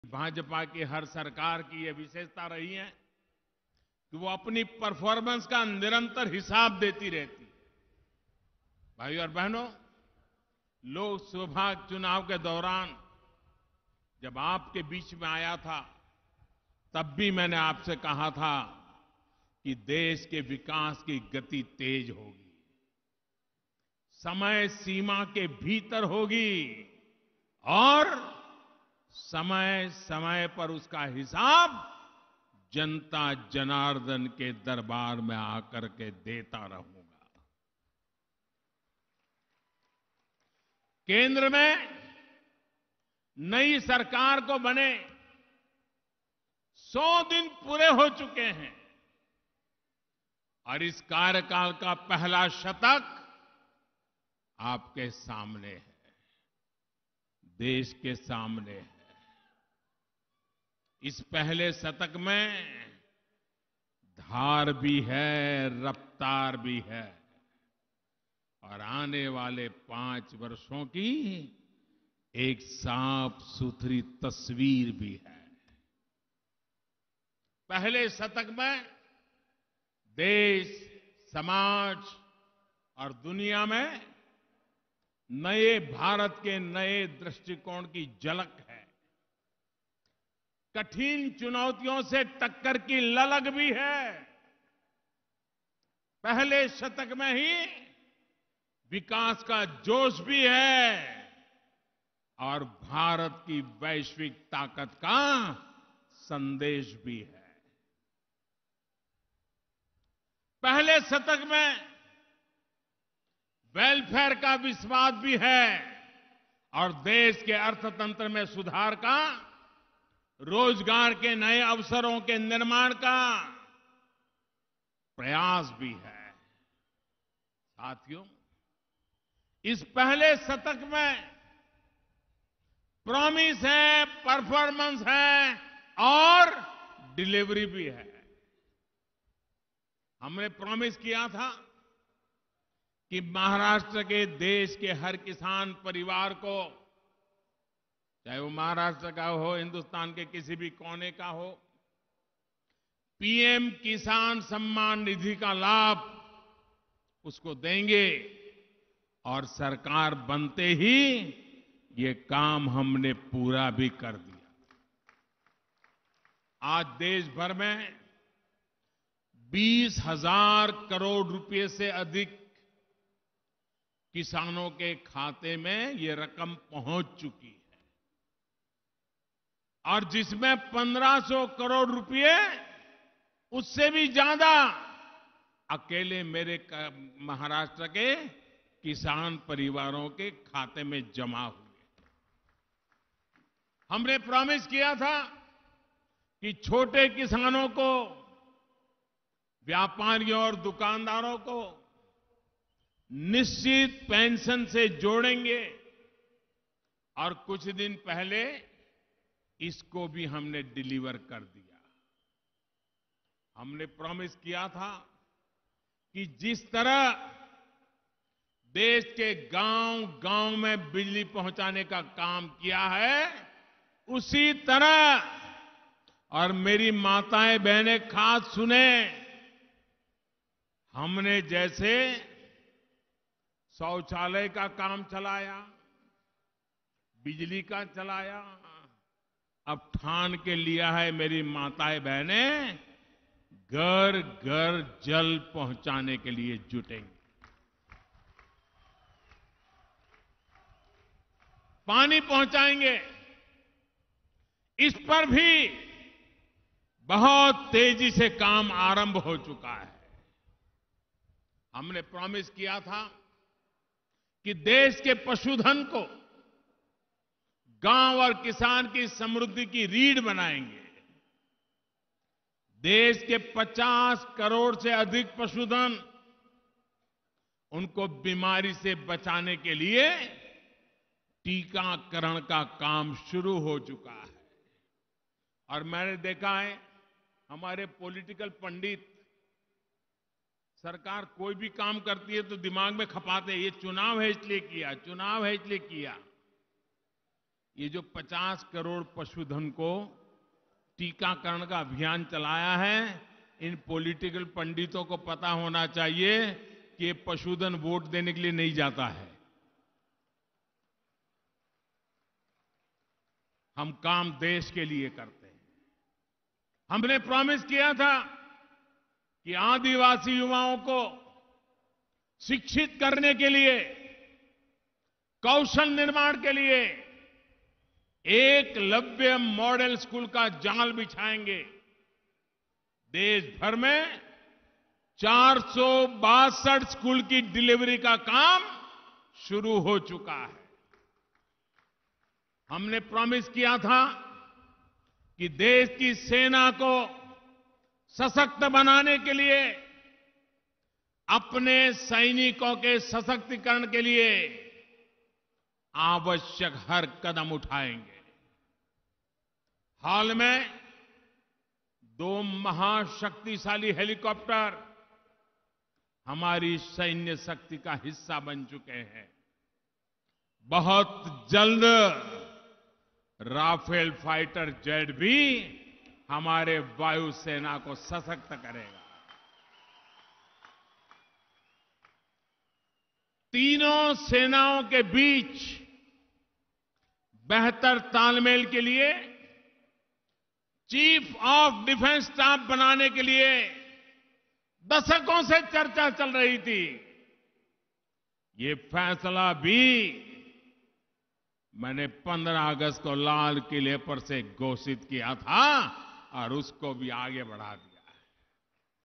Promise, Performance & Delivery...
Prime Minister Narendra Modi addresses a public meeting in Nashik, Maharashtra